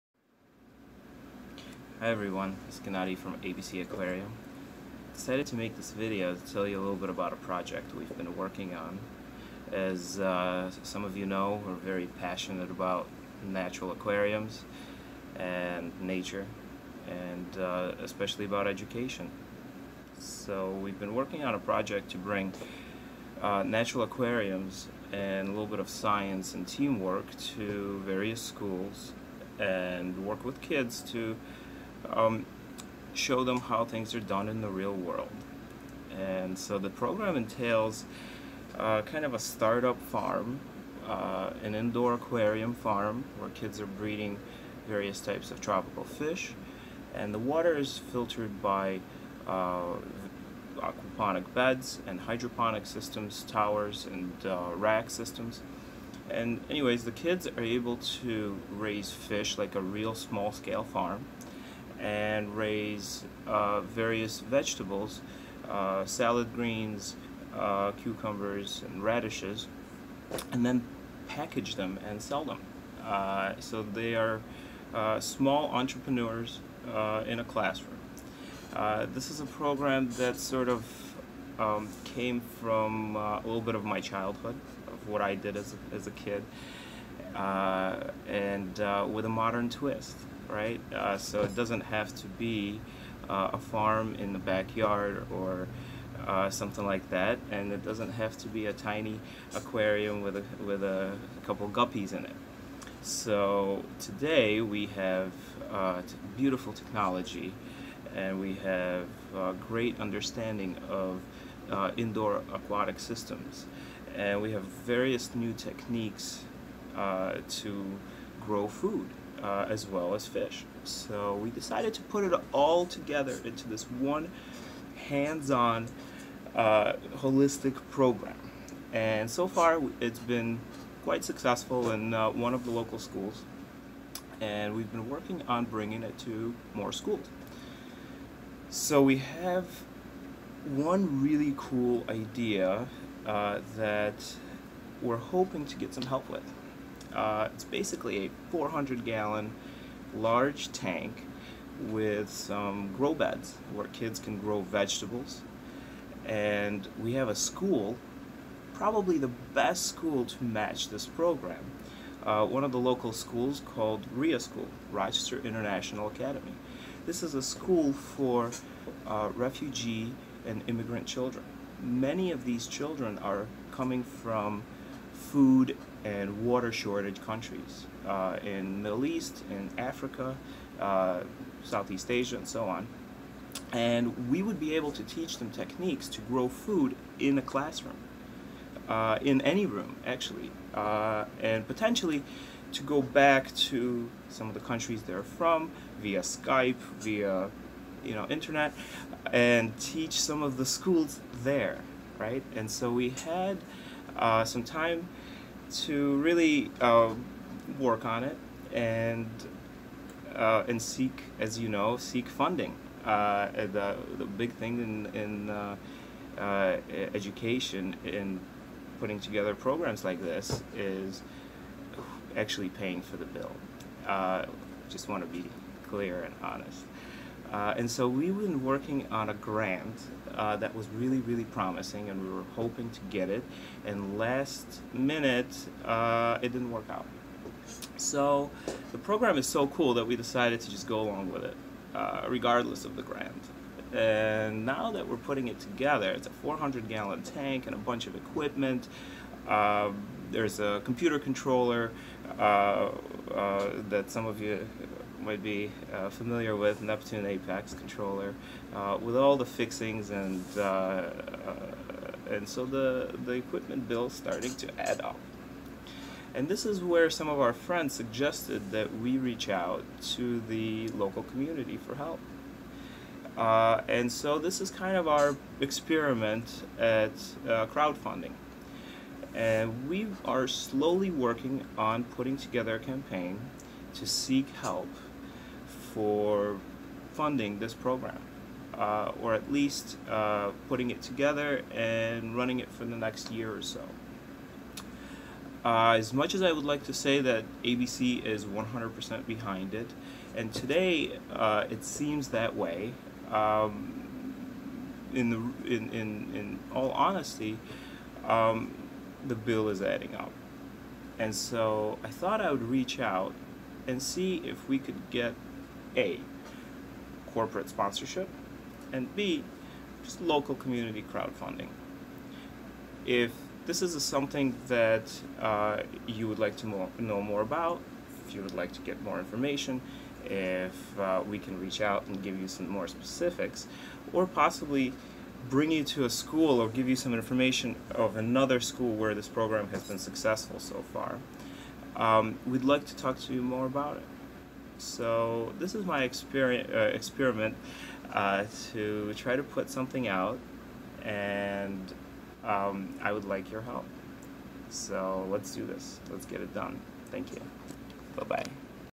Rochester NY RIA School Project.
We (ABC Aquarium) have been working on developing a hands-on educational program to teach children about nature, science, sustainability, healthy eating, farming, business, ethics, team work and much more.

This project has been quite successful in our first school, and now we need help to bring the program to another school.

Our goal is to install a large Aquarium / Aquaponic system in Rochester International Academy in 2019.

For information regarding the program or details about this campaign please reach out to Gennadiy M.